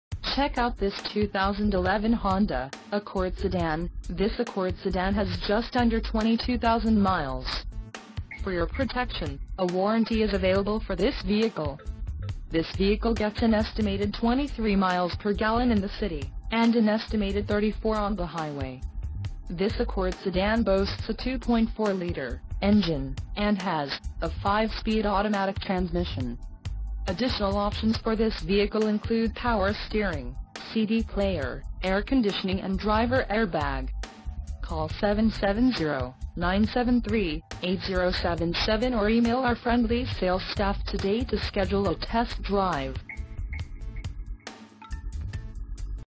2011 Honda Accord Sdn 4dr I4 Auto LX Sedan - Marietta, GA
Marietta Auto Sales, INC, Marietta, GA - CLEAN AUTOCHECK! CD PLAYER, CRUISE CONTROL, REAR DEFROSTER & MORE!!!